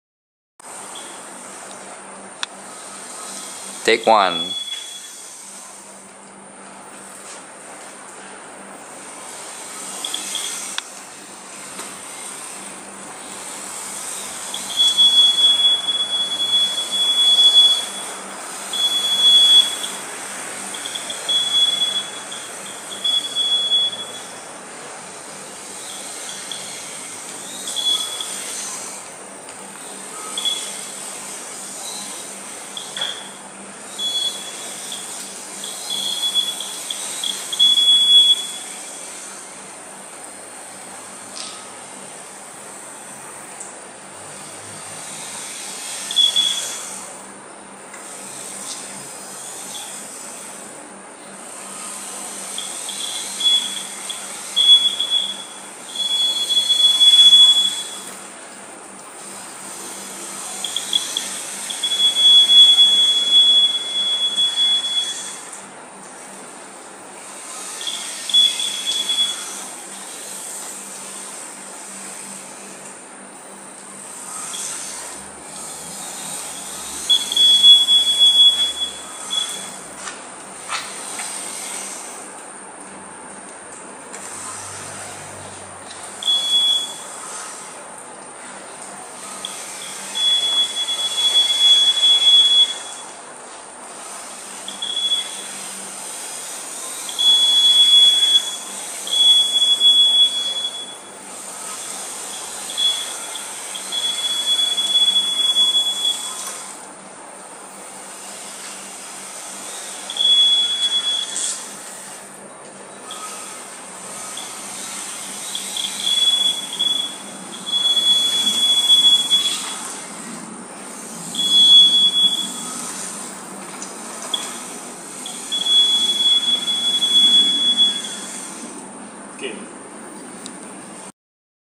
Self balancing scooter second run
PID tuned further and added max speed buzzer